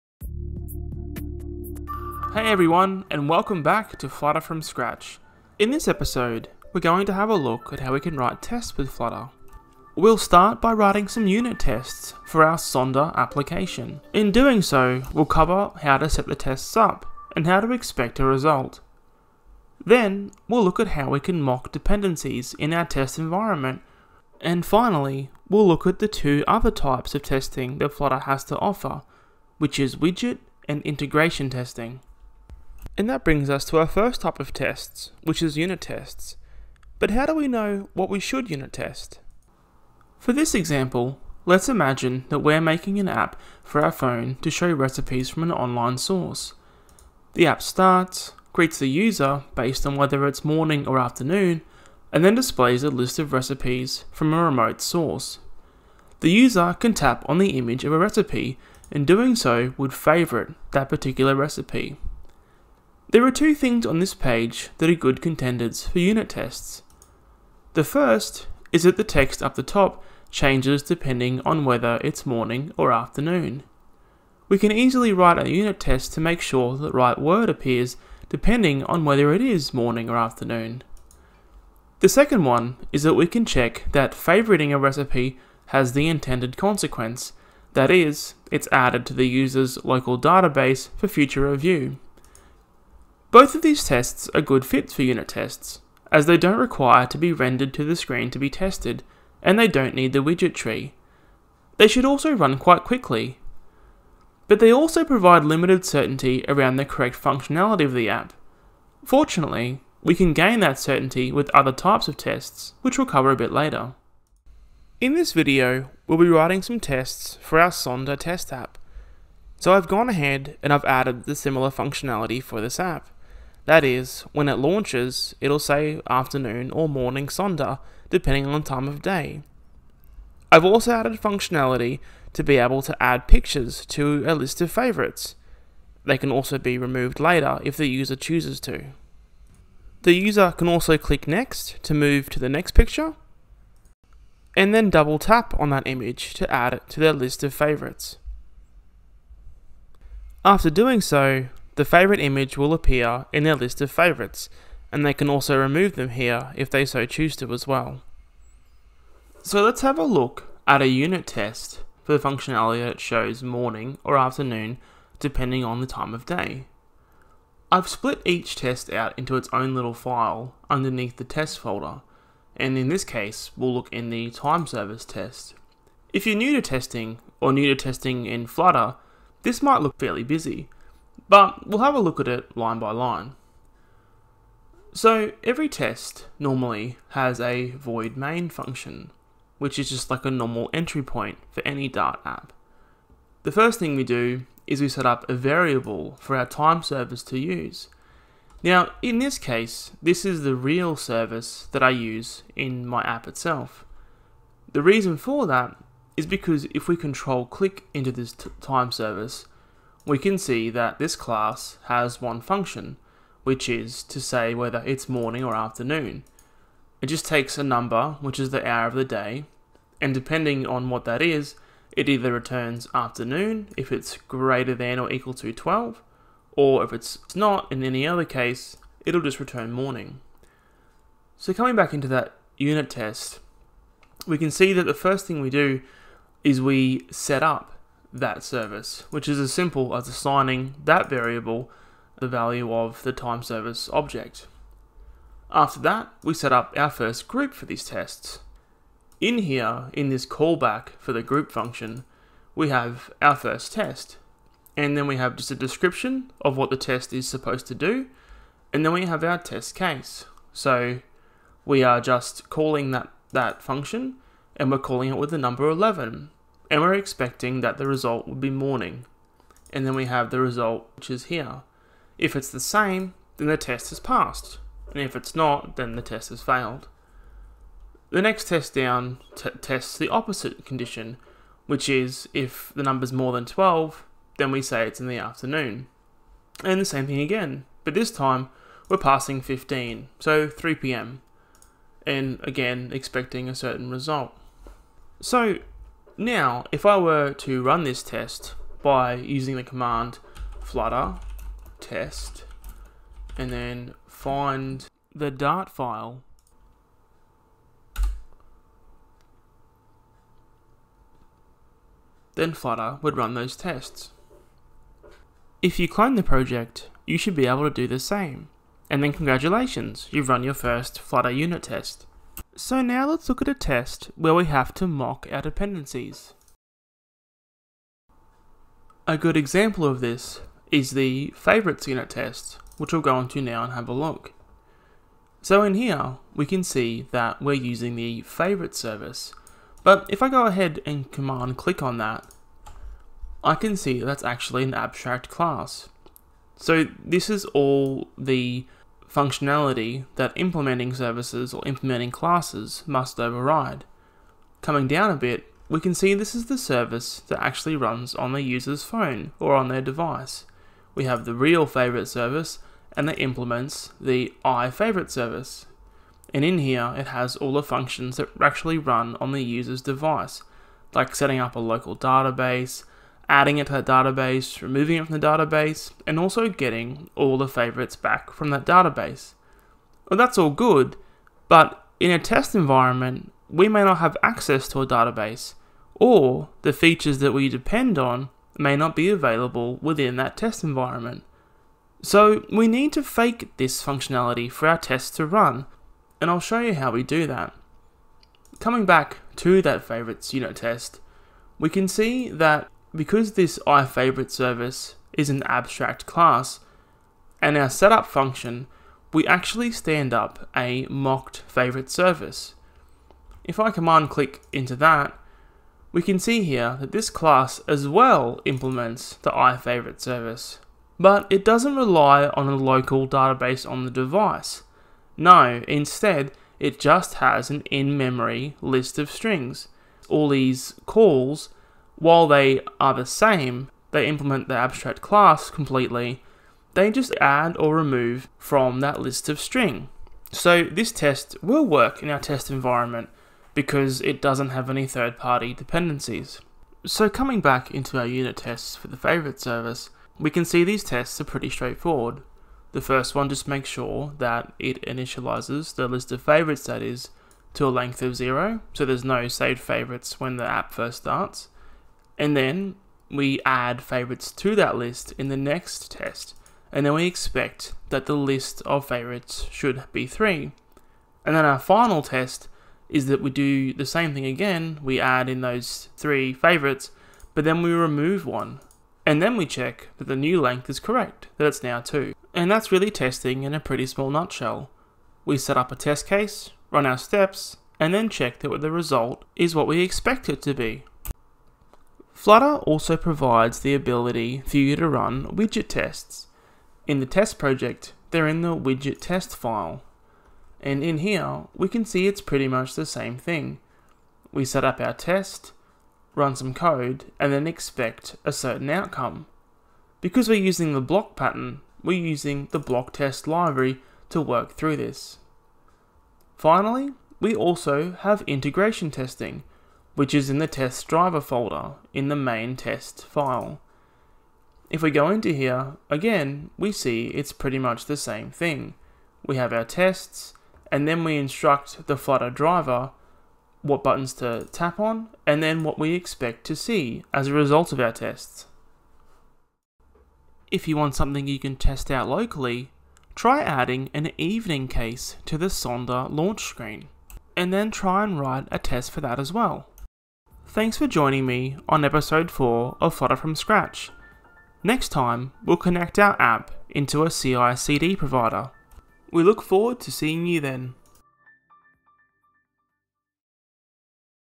Episode 4: Testing with Flutter
Welcome back for the fourth episode of Flutter From Scratch! In this episode, we take a look at how to effectively test our Flutter application. We start to understand what we should unit test, how to mock our dependencies, and also take a look at Widget and Integration testing. High quality tests means high quality apps!

🔬🔬🔬PREREQUISITES:🔬🔬🔬
▶Please make sure you watch the third episode about Flutter widgets and states before watching this episode. Third episode

🐯Episode 4:🐯

[0:39] - What should we unit test?
[1:54] - What we will test in Sonder
[2:51] - Our first simple unit test
[5:11] - Running the unit test
[5:26] - Mocking dependencies for our second unit test
[5:42] - Using abstract classes to help mocking
[7:29] - Unit test using a mocked service
[8:12] - Overview of other types of testing

💥💥💥CHALLENGE💥💥💥

Can you make the app show ‘Evening Sonder’ in addition to it’s two current ‘Morning’ and ‘Afternoon’ cases? And better still, can you write a test that passes after doing so?

🎈If you have any questions or want something to be covered in the upcoming episodes, be sure to let us know on the Codemagic Slack channel.